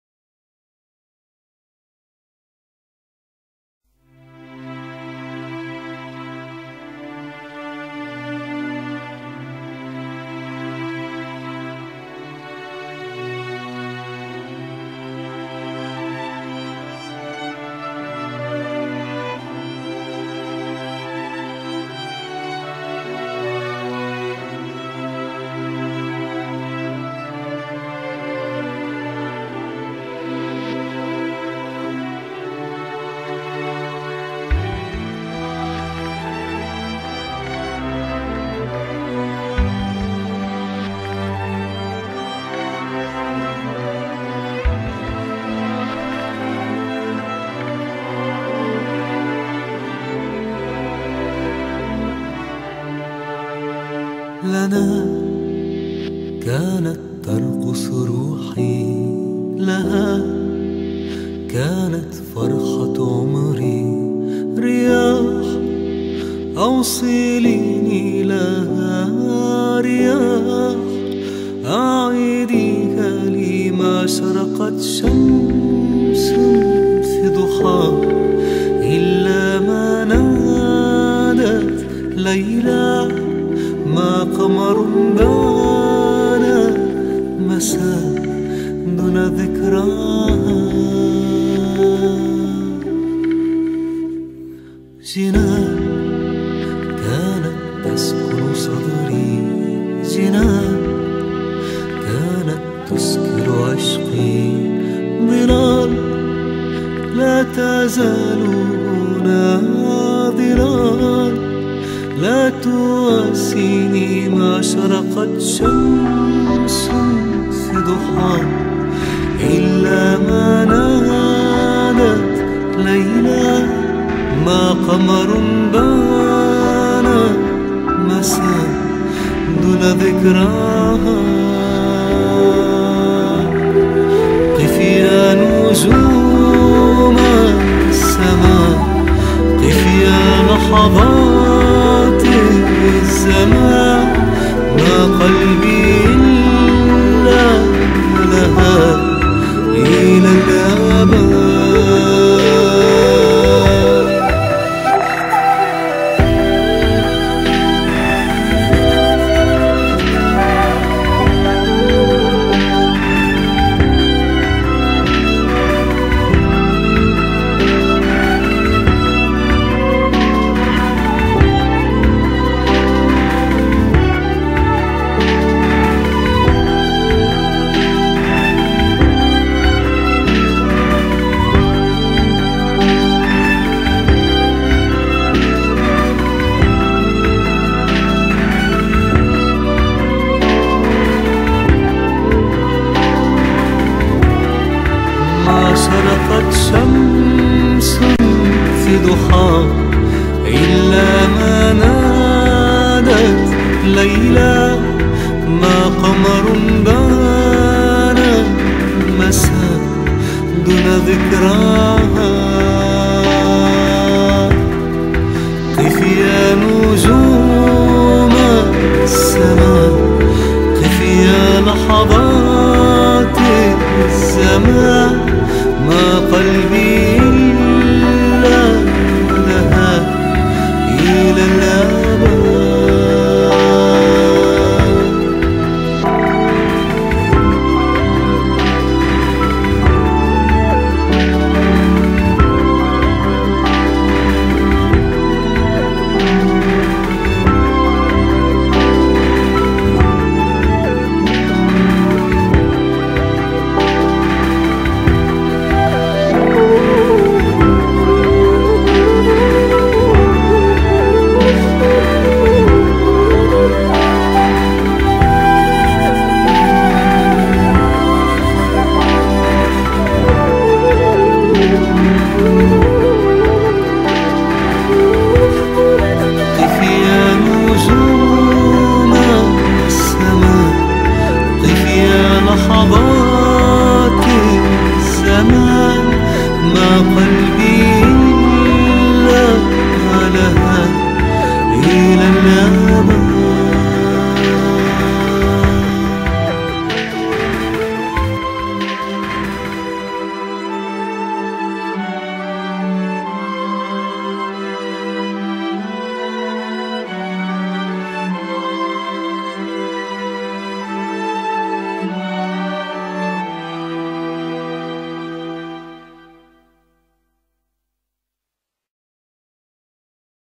La triste storia di Layla e Majnun
Una classica storia araba di un amore contrastato, nota anche come "Il Folle e Layla".